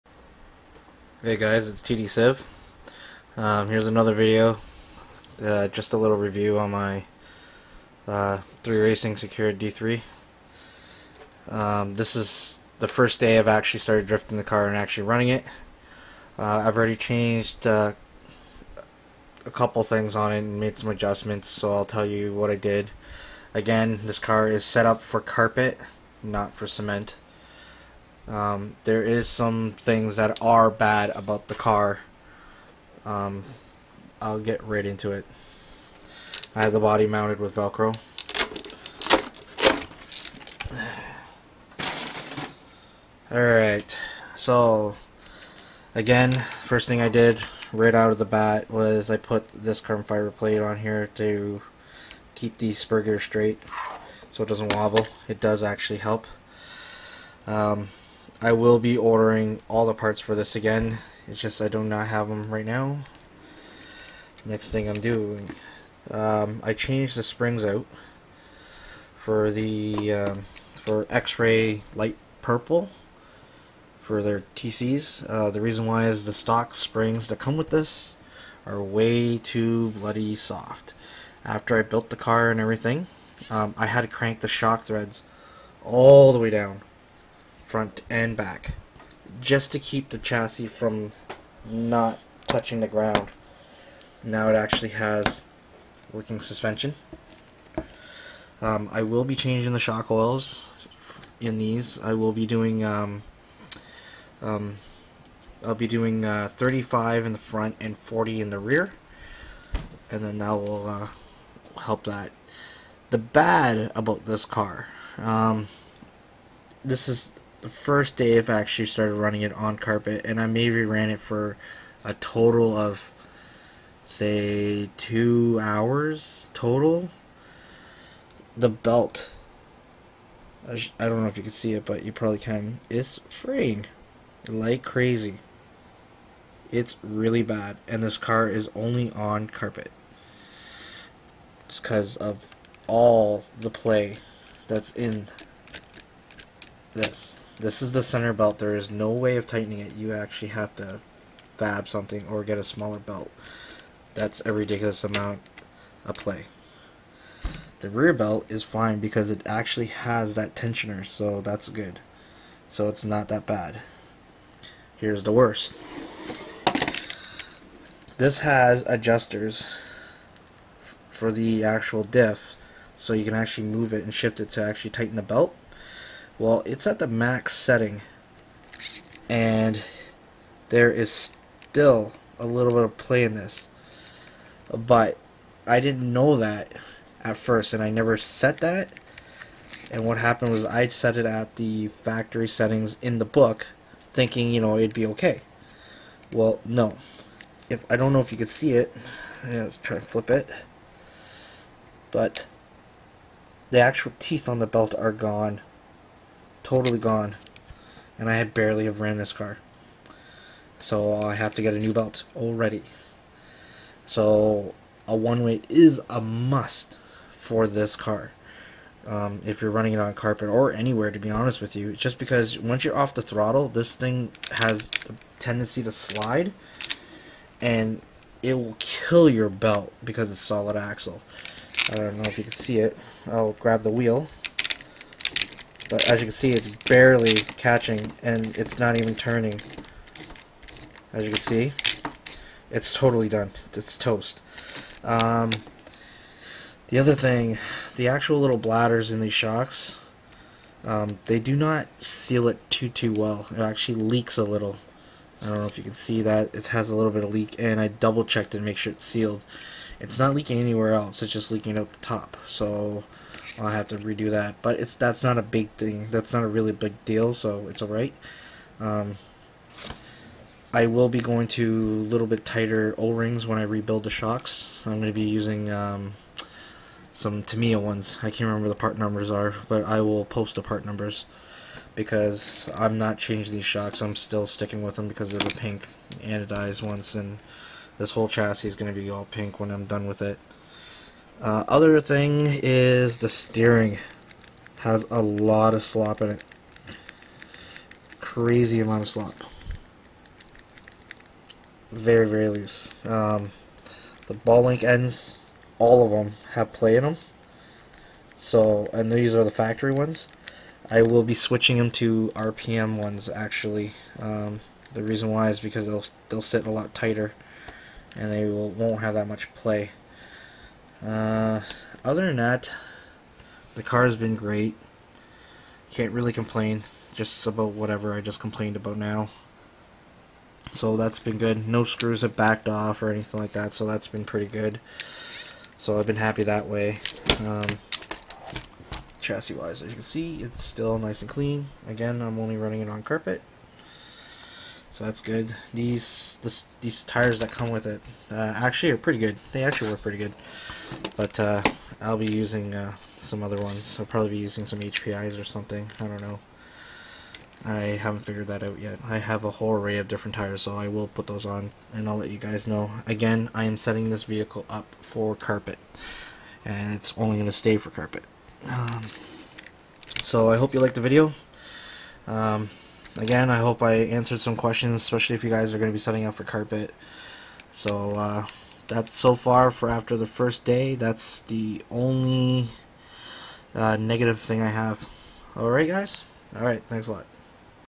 3Racing Sakura D3 CS over look part 2
here is another over look in to my 3Racing Sakura D3 CS

still a great but but there is some things im not happy with

1 stock spring suck and i already changed them to X-ray light-purple springs
2 already need to change my front belt and i only have about 2hr of run time on this chassis
3 too much slop in the steering rack
4 the stock ball links ends have too much slop in them im going to changed them to rpms
5 center belt has too much slop in it im going to make a custom belt tensioner for it
6 the stock belts are very cheap and dont last ill be looking in to better belts and let you know what i find